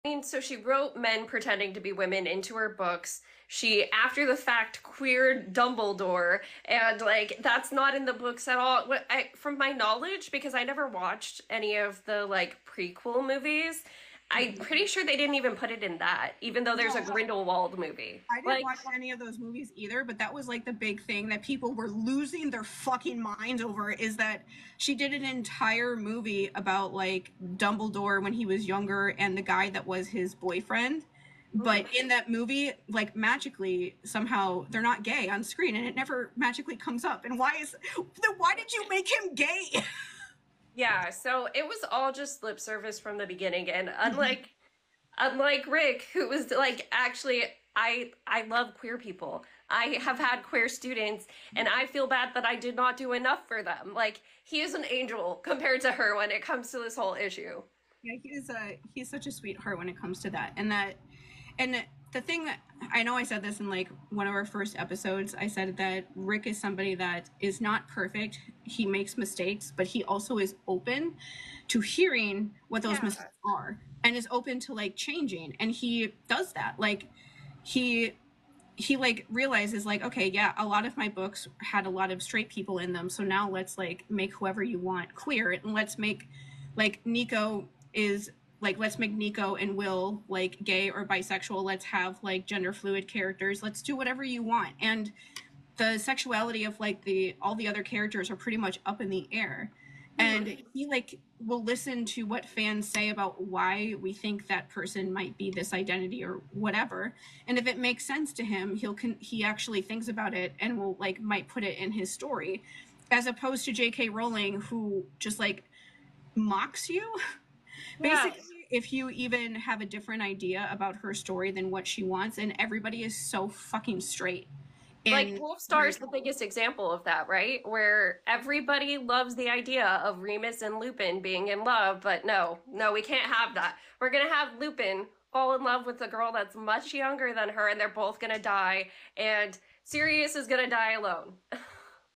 JK Rowling vs Rick Riordan’s approach to queer characters
JKR’s view of her characters’ queerness has always been flawed, yet Rick Riordan continues to try his best to depict lgbtq+ characters.